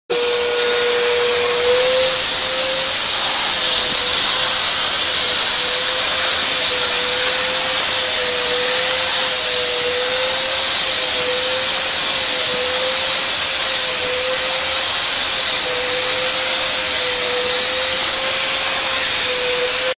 significant cleaning effect with 6kw steam vacuum cleaner to clean carpet
2.We are manufacturer of steam car wash machine,household steam cleaner,high pressure cleaning equipment with 11 years experience.Good quality and competitive price.